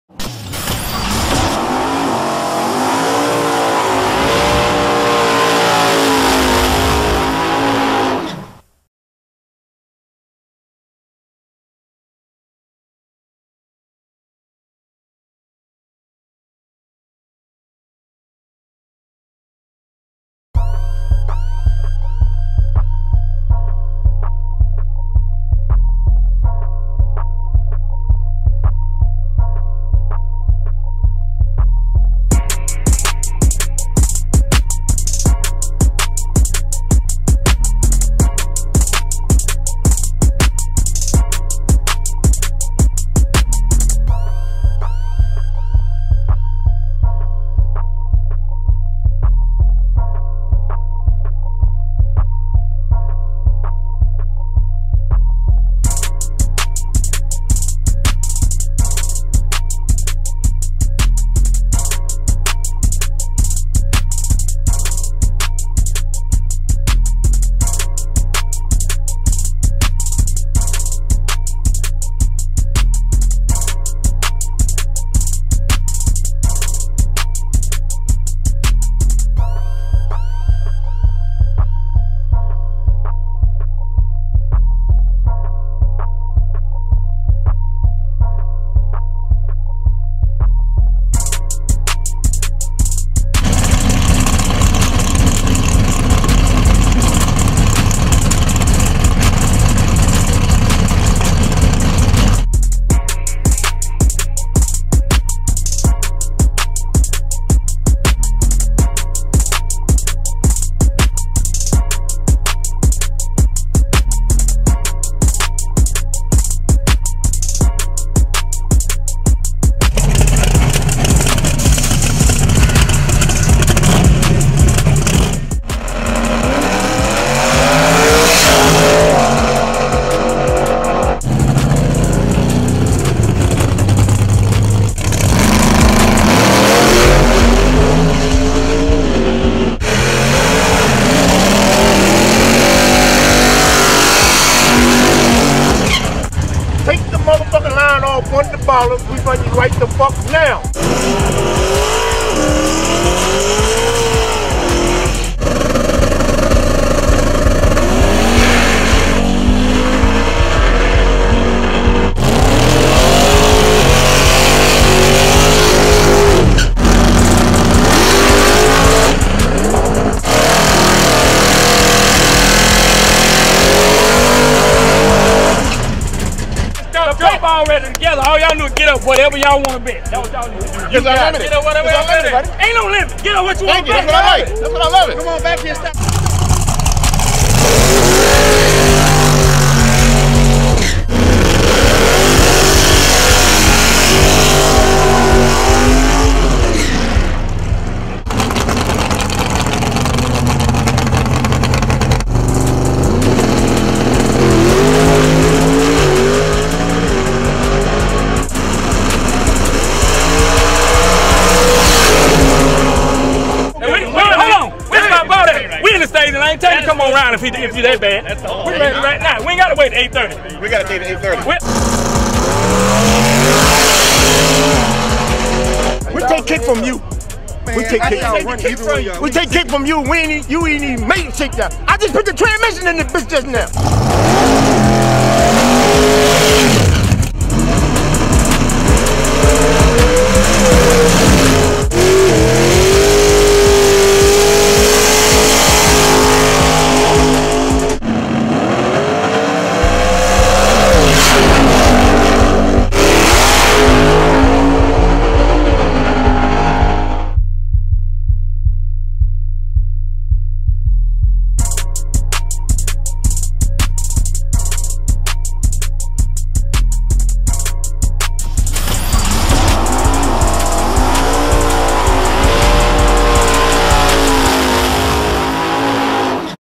Nyce1s - Custom T's & L.I. Joe's Grudge Finale October 16th & 17th, 2015...
L.I. Joe and Custom T's are bringing you their grudge race event at Fayetteville Motorsports Park on Friday October 16th and Sunday October 17th, 2015…

Check us out on Instagram @officialnyce1s